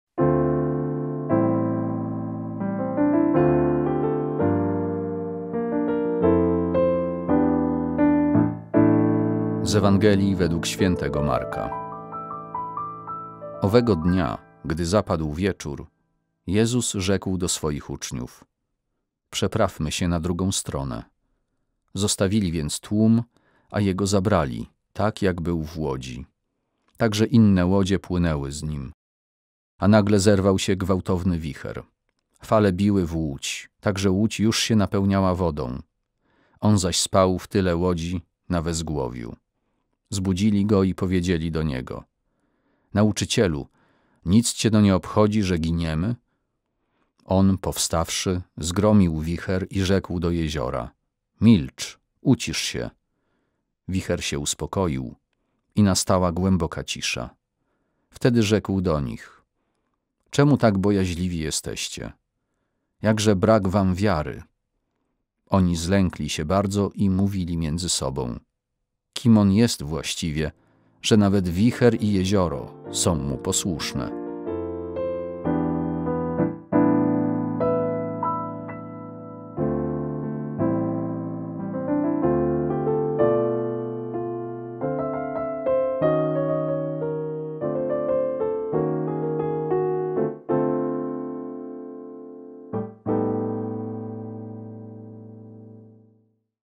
Ewangelia | 20 czerwca 2021
Owego dnia, gdy zapadł wieczór, Jezus rzekł do swoich uczniów: «Przeprawmy się na drugą stronę». Zostawili więc tłum, a Jego zabrali, tak jak był w łodzi. Także inne łodzie płynęły z Nim.
A nagle zerwał się gwałtowny wicher. Fale biły w łódź, tak że łódź już się napełniała wodą. On zaś spał w tyle łodzi na wezgłowiu. Zbudzili Go i powiedzieli do Niego:
«Nauczycielu, nic Cię to nie obchodzi, że giniemy?» On, powstawszy, zgromił wicher i rzekł do jeziora: «Milcz, ucisz się!» Wicher się uspokoił i nastała głęboka cisza.
Wtedy rzekł do nich: «Czemu tak bojaźliwi jesteście? Jakże brak wam wiary!» Oni zlękli się bardzo i mówili między sobą: «Kim On jest właściwie, że nawet wicher i jezioro są Mu posłuszne?»

Montaż: ś.p. Bogdan Głowacki.